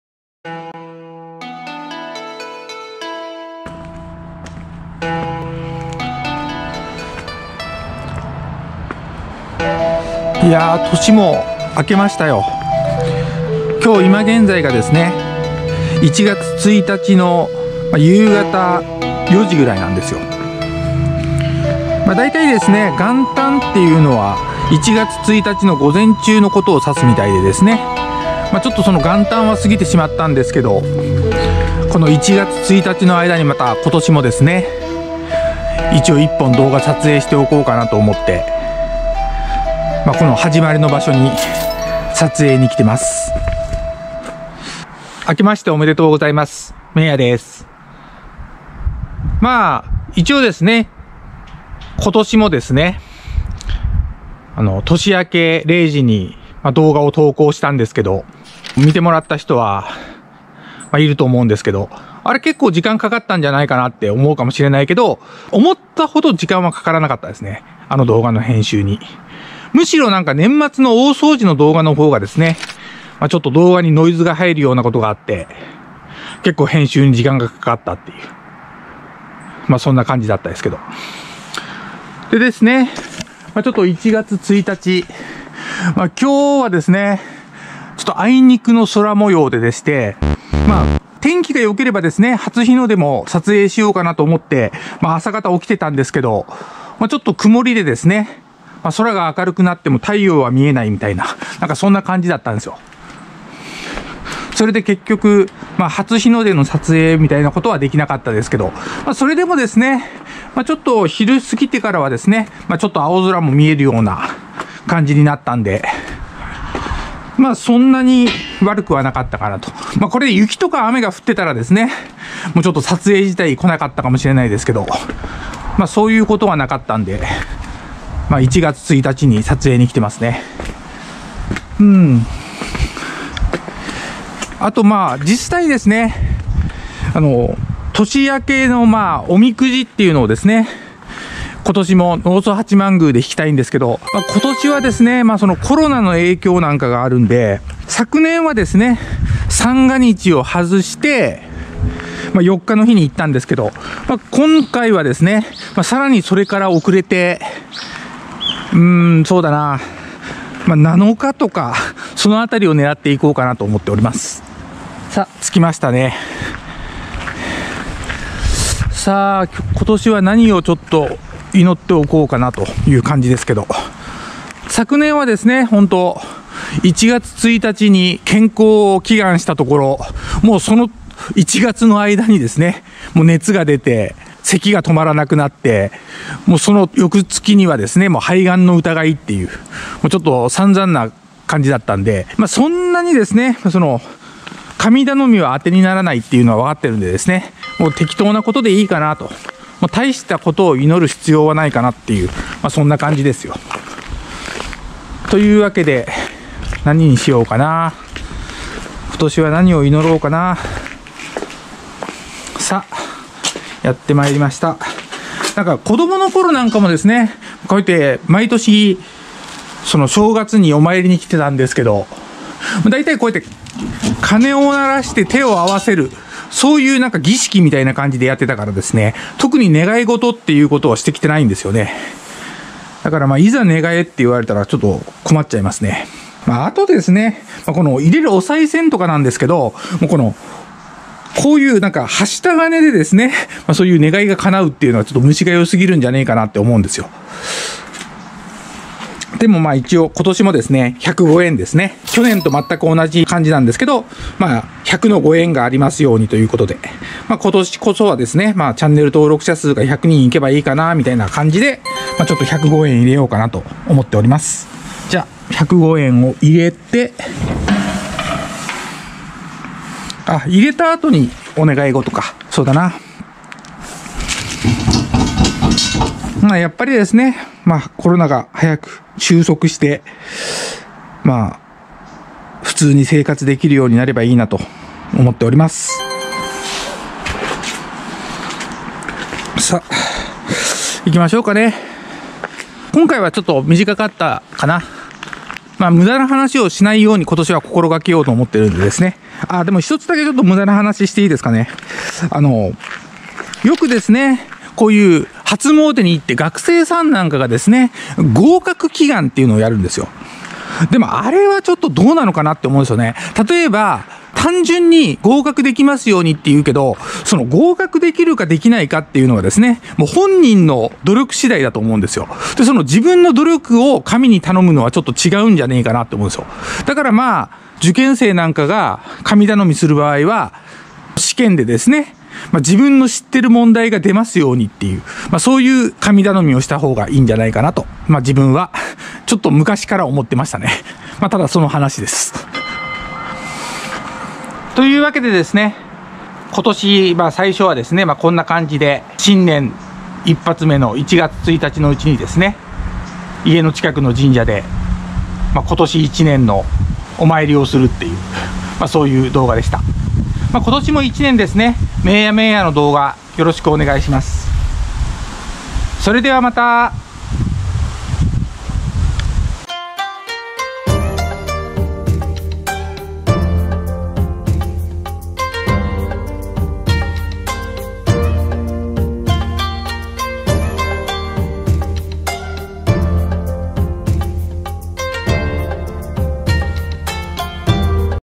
【２０２１初詣】今年も宜しくお願いします。
今年は元日、あまり天気がよくありませんでしたが、
一年の始まりということで初詣に行って来ました。
※今年も動画投稿頑張っていこうと思います。(゜_゜)